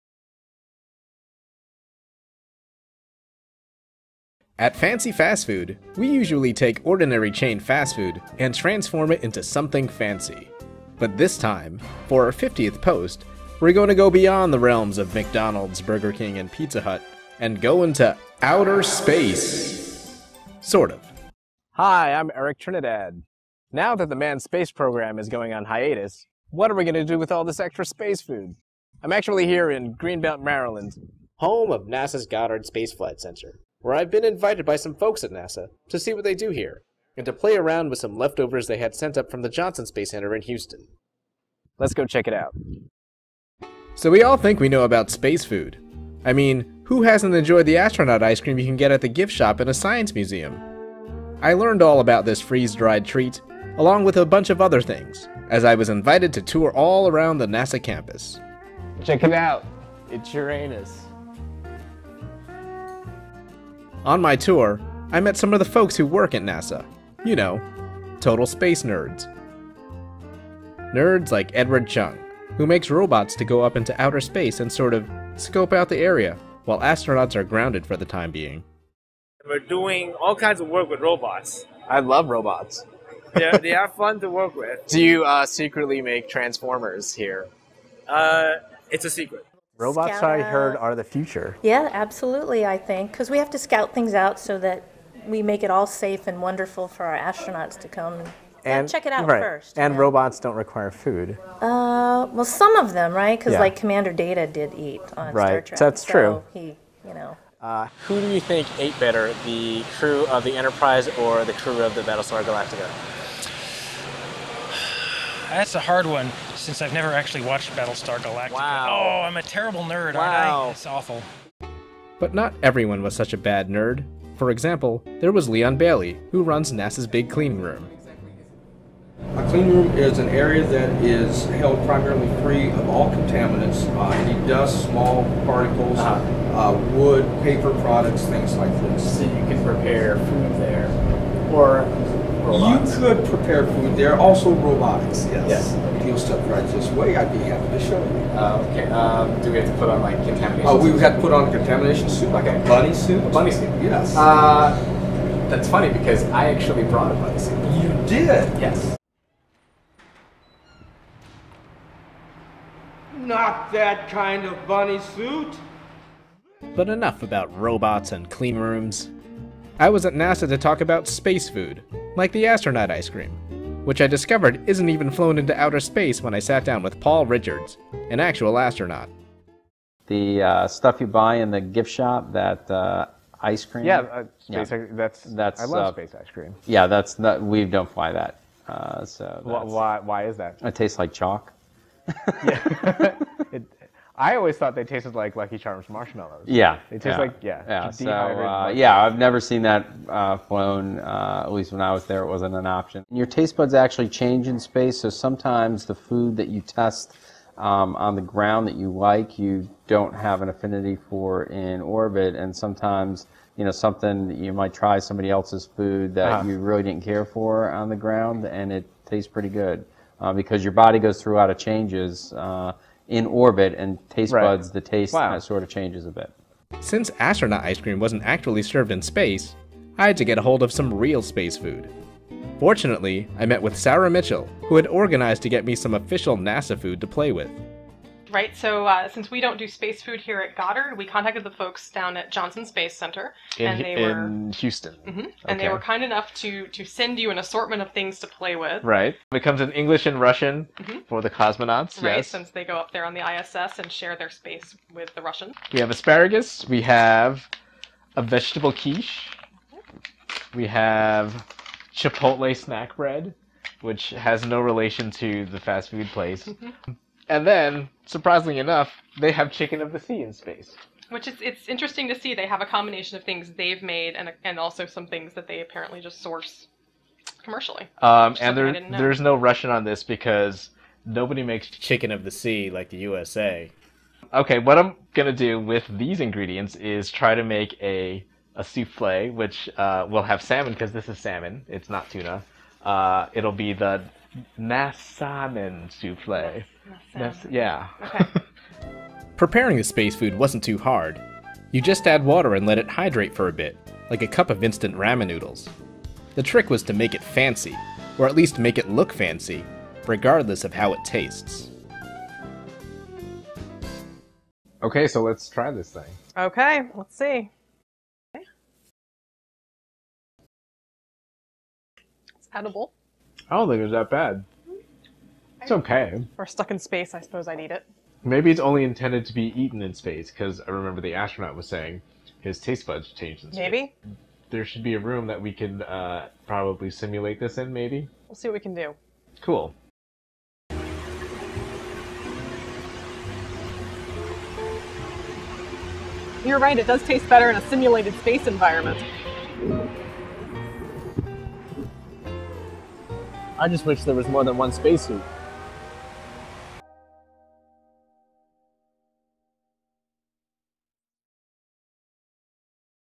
Making Fancy Space Food at NASA
Fancy Fast Food creator Erik R. Trinidad tours NASA's Goddard Space Flight Center, before attempting to make leftover astronaut food look fancy — now that the U.S. manned space program has been put on hiatus.  (Produced by Erik Trinidad, Maggie Masetti, Sara Mitchell, and Maurice Murdock.)

(The views and opinions in this video are not necessarily shared by NASA, nor do they reflect any of their official policies.)